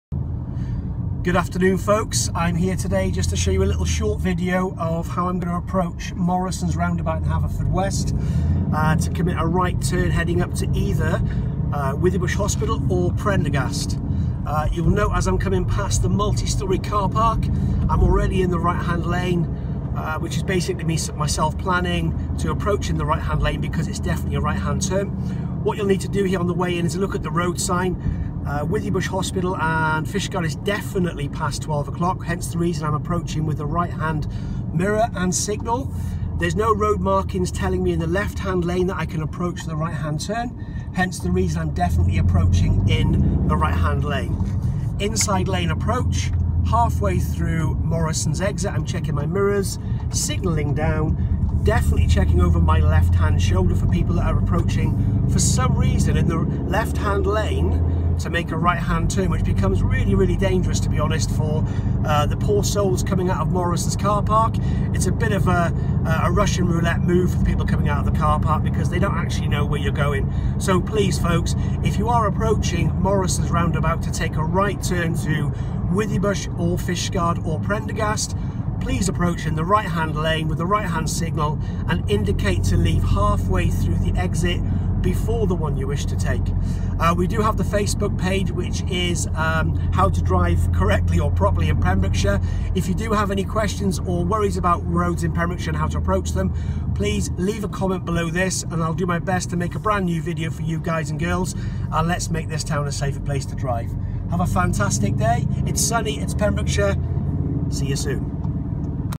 How to correctly approach Morrisons roundabout in Haverfordwest!
Fast-Track Driving School's aims to make the general public aware of how to correctly approach the roundabouts in the town of Haverfordwest.